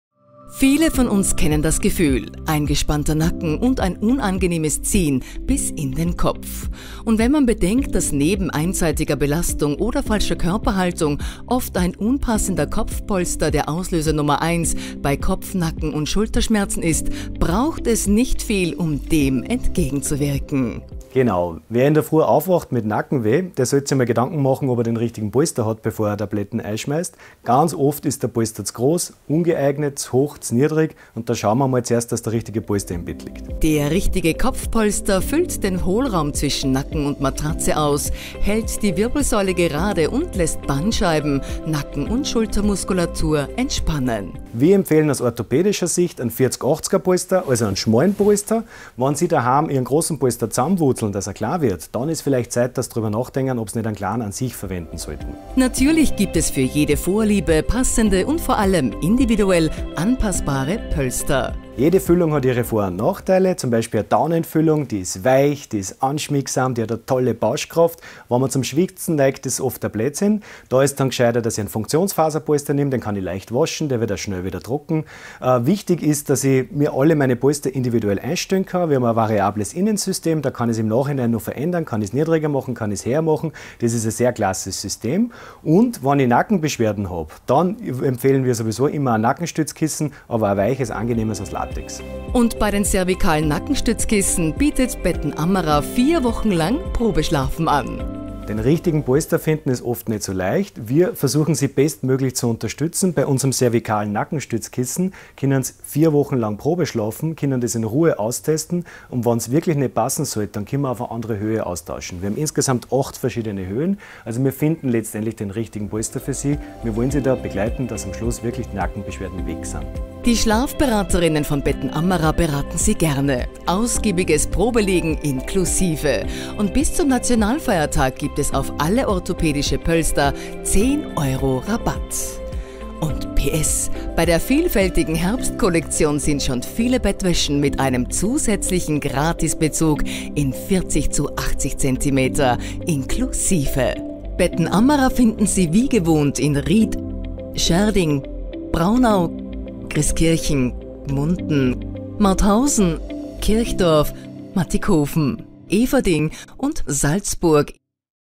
Nackenschmerzen? Ein richtiger Polster lässt Nacken, Bandscheiben und Schultermuskulatur entspannen.
Wer morgens mit Nacken- oder Schulterschmerzen aufwacht, sollte einmal seine Schlafgewohnheiten und vor allem den Kopfpolster genauer unter die Lupe nehmen. Die Schlafberaterinnen von Betten Ammerer beraten Sie gerne! Jetzt ist Polsteraktion bei Ammerer (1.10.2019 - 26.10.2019) auch